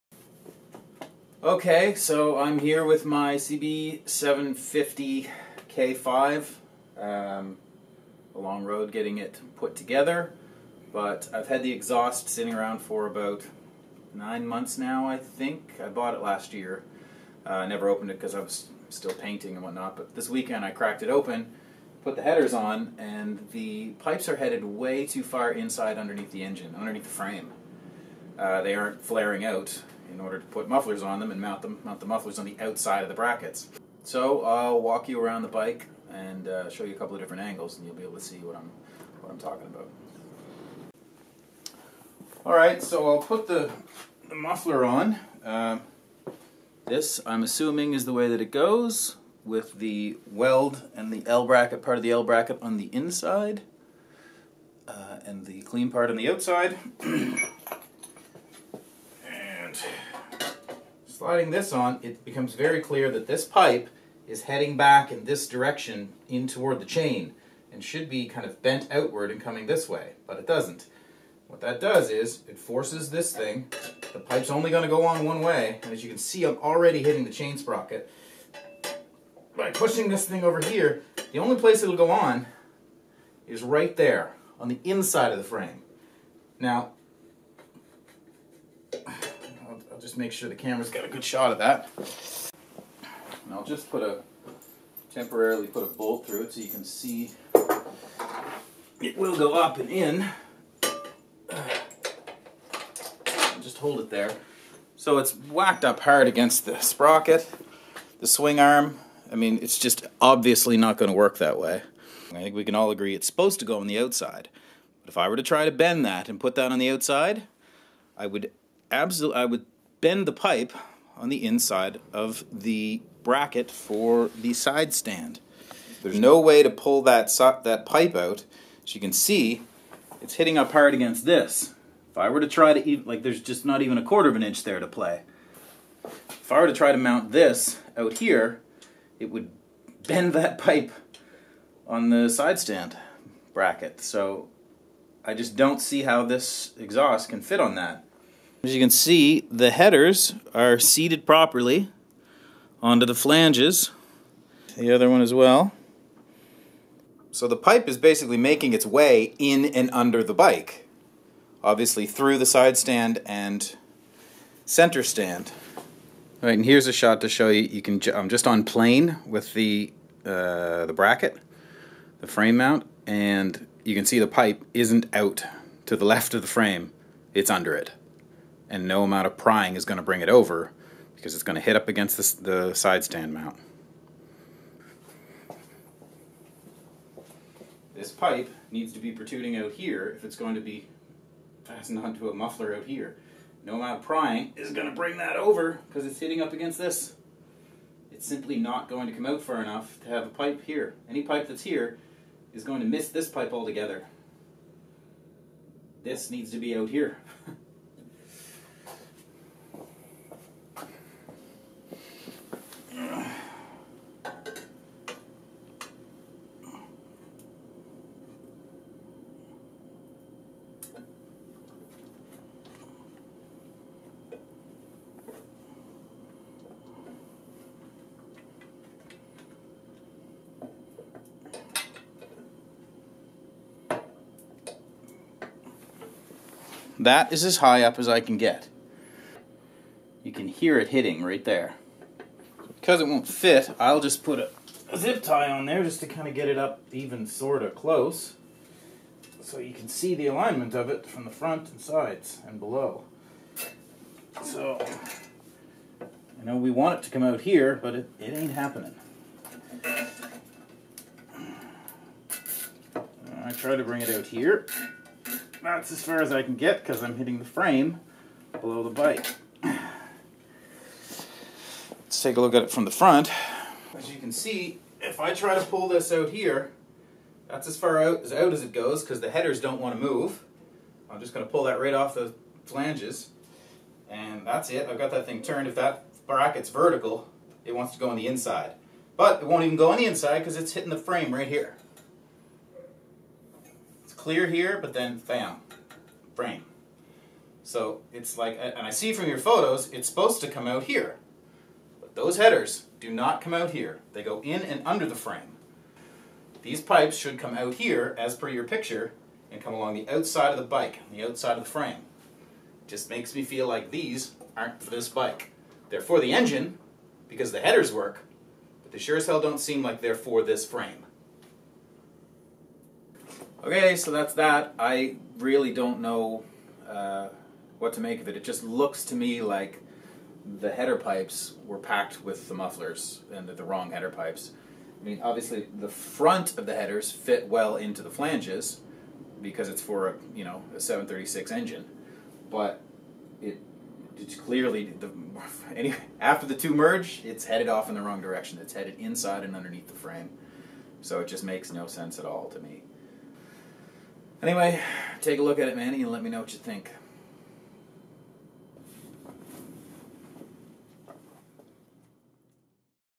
CB750K5 Mac Performance 4 into 2
Hi Manny,
Take a look. I'm kind of at a loss. I ran the serial number of the frame. It's a K5 frame (as the sticker says).  The bike is stock in every way. Factory centre stand, side stand, etc..
I feel like someone may have packed the wrong headers into the box.
I'll call you Tuesday to see what you make of it.
Thanks Man.
Bob